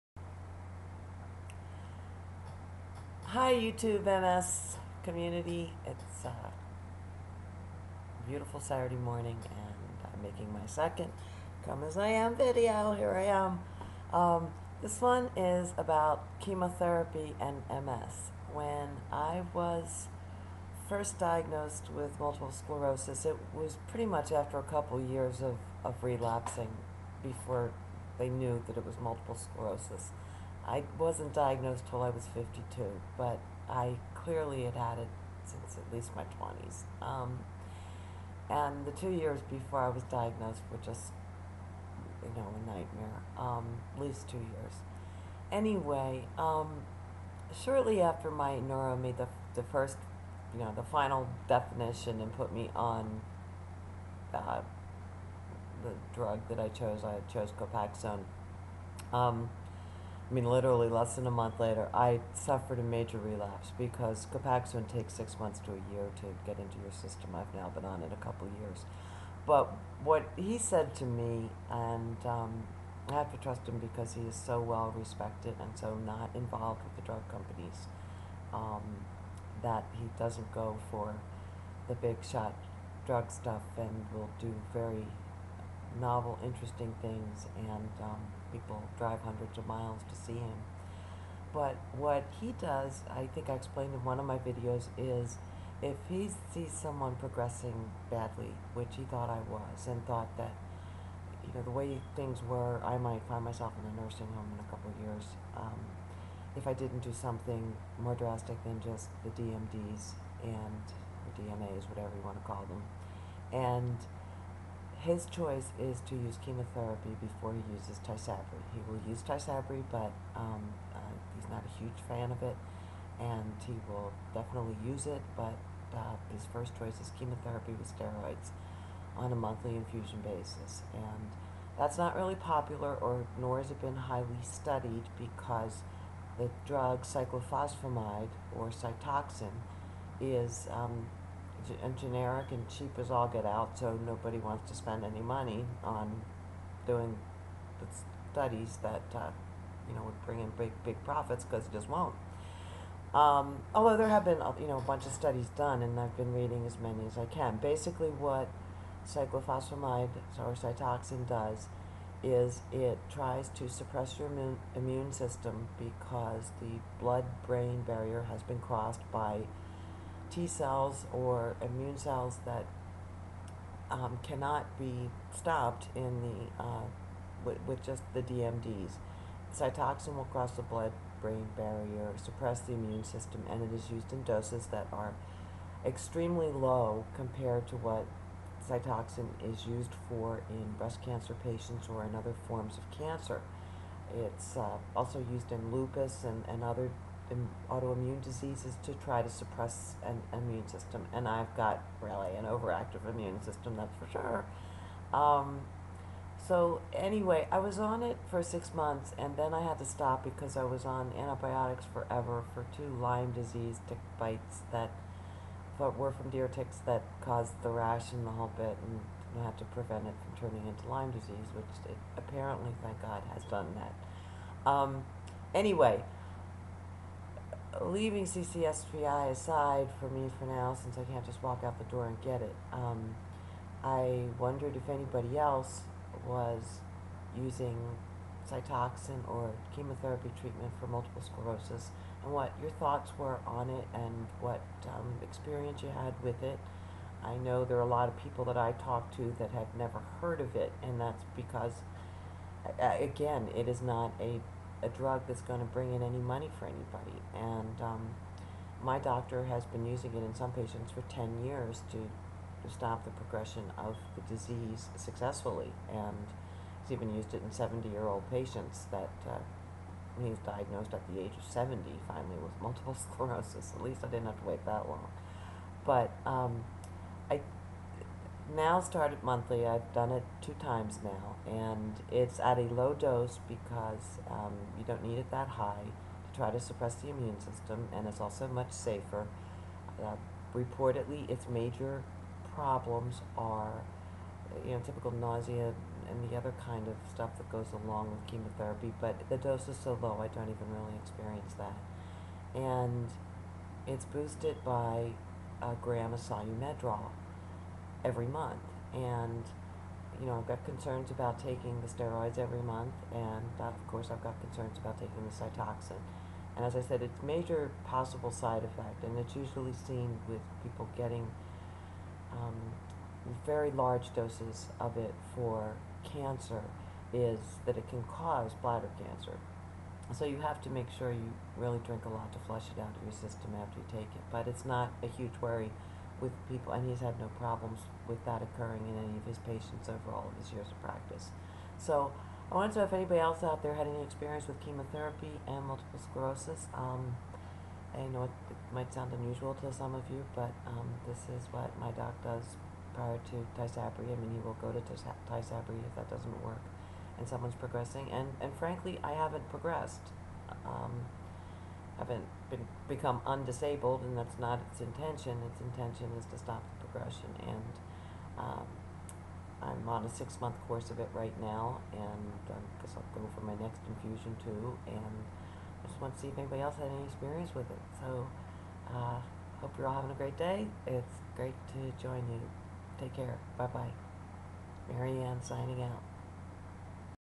MS and Chemotherapy
I take cytoxan and steroid infusions monthly to stop progression